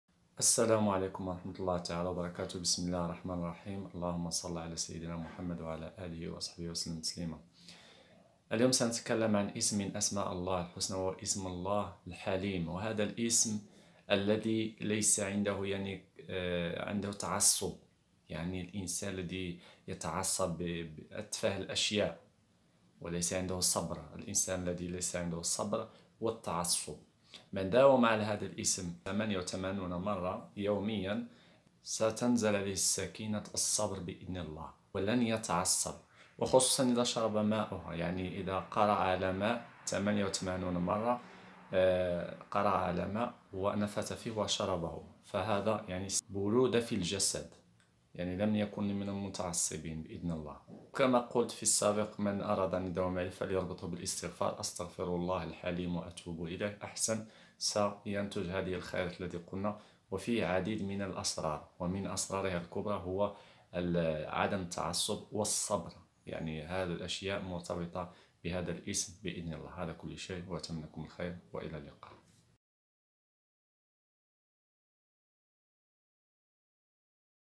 من داوم على هذا ذكر كل يوم 88 مرة ألهمه الله تعالى الصبر وكظم الغيظ و خرج من قلبه الحسد والكبر
من داوم على ذكر كل يوم 88 مرة ألهمه الله تعالى الصبر وكظم الغيظ و خرج من قلبه الحسد والكبر